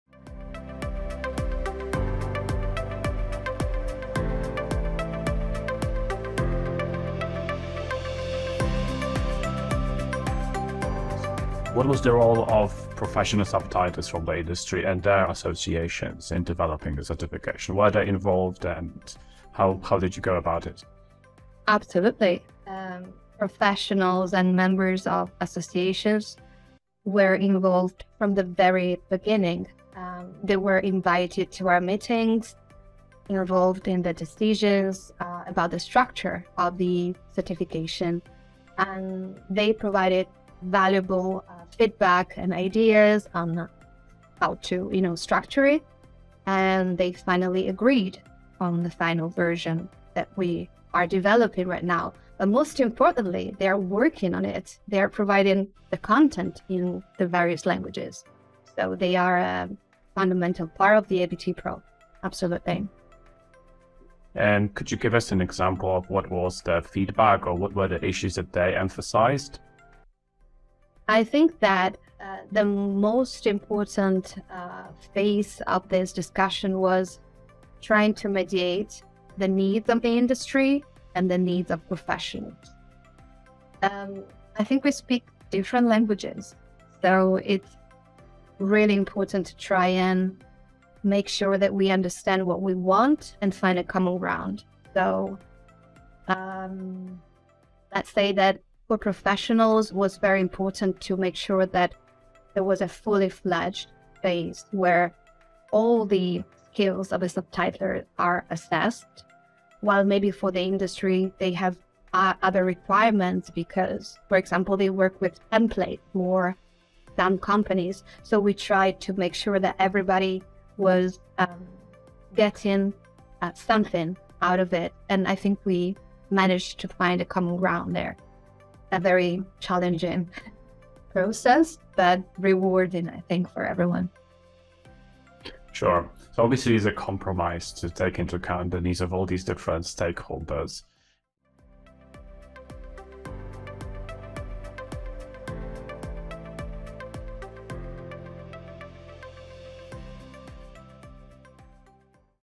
THE ROLE OF SUBTITLERS IN DEVELOPING AVT PRO - Interview with Dr Serenella Massidda
Łukasz Dutka interviews Dr Serenella Massidda, a prominent figure behind the AVT Pro certification in subtitling.